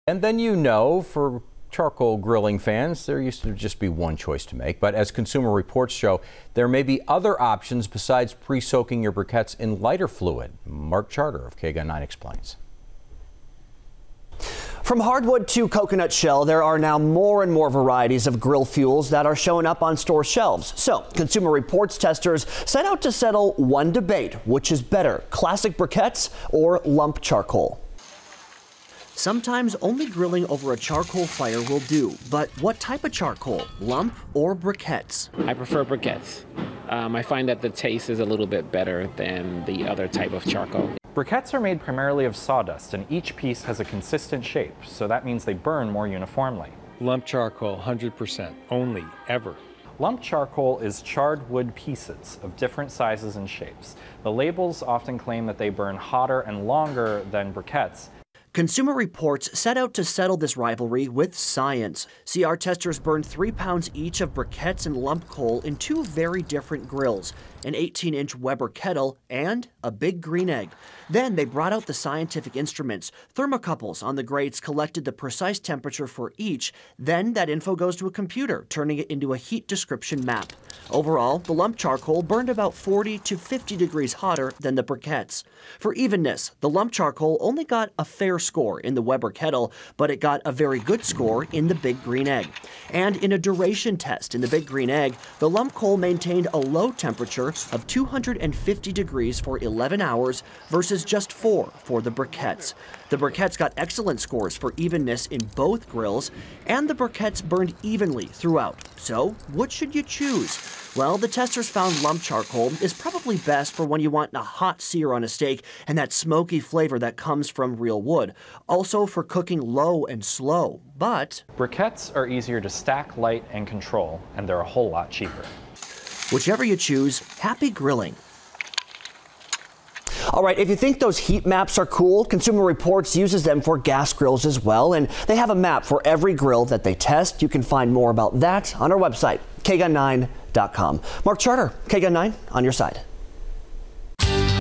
Consumer Reports: Charcoal V. Briquettes
Sometimes, only grilling over a charcoal fire will do! But what type of charcoal? Lump or briquettes?